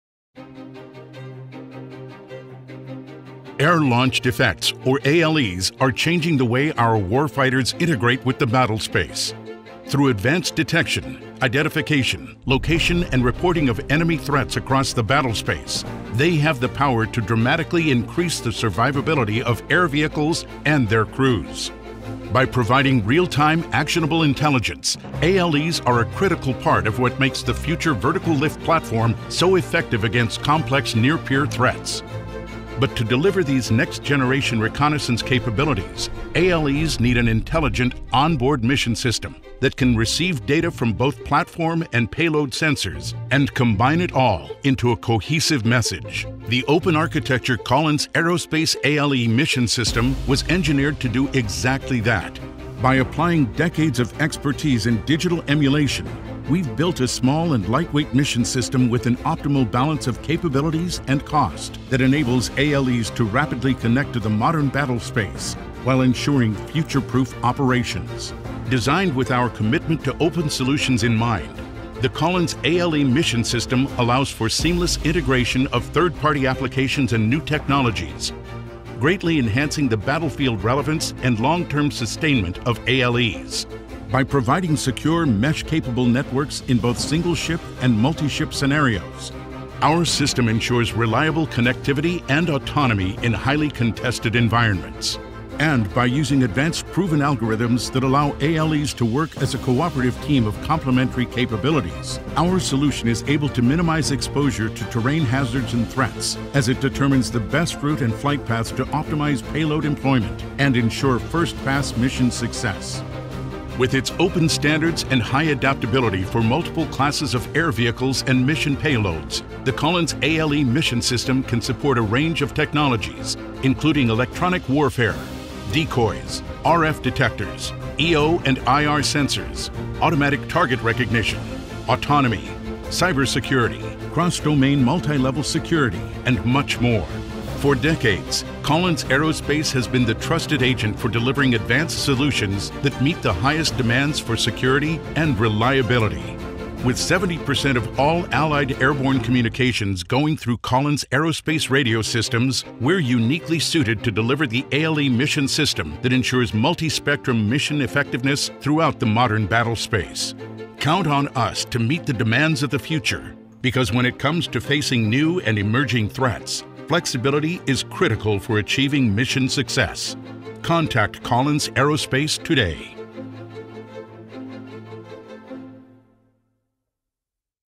Mission System for Air Launched Effects (ALE)
Collins’ small, lightweight and open-architected Mission System enables ALEs to rapidly and securely connect to the modern battlespace. Through advanced fusing of time-critical targeting data derived from air vehicles, payloads and networked sensors; our solution maximizes manned-unmanned teaming and advanced autonomy to deliver a cohesive and accurate message that dramatically increases the survivability and mission effectiveness of high-value air assets and their crews in any Anti-Access/Area Denial (A2/AD) environment.